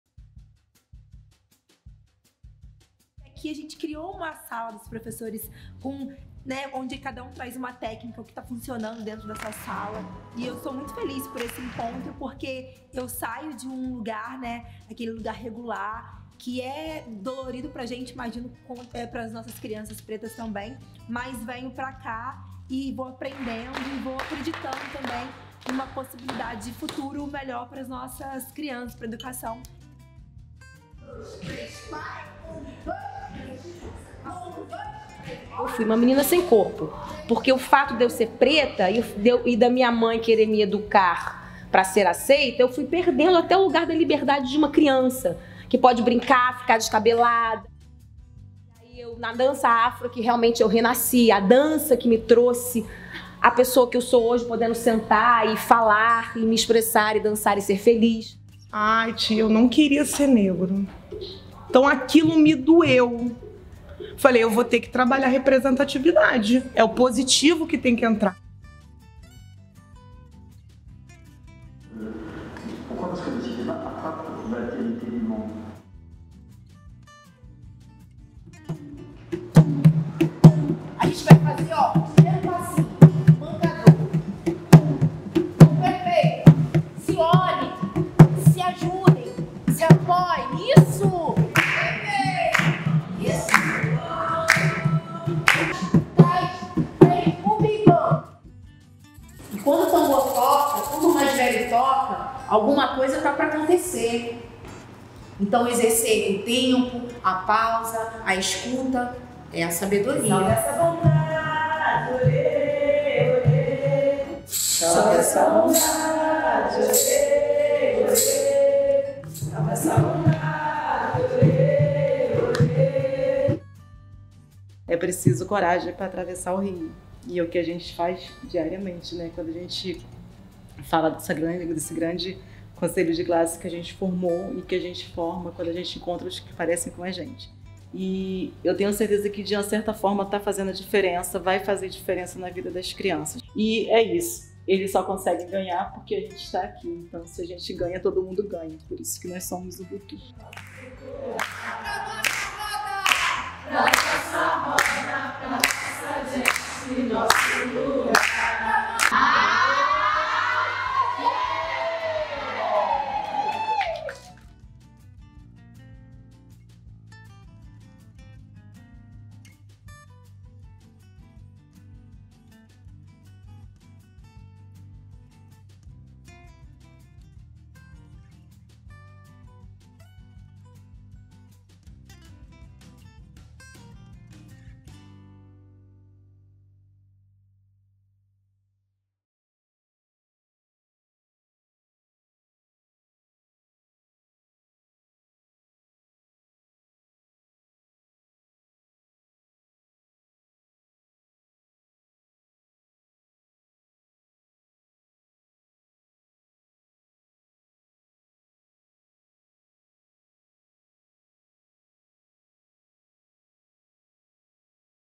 Formação de Professores em Artes Integradas | PIM 2022
Formação oferecida pelo Programa Imagens em Movimento para professores da rede pública de ensino do Rio de Janeiro

Pílula Audiovisual 1/5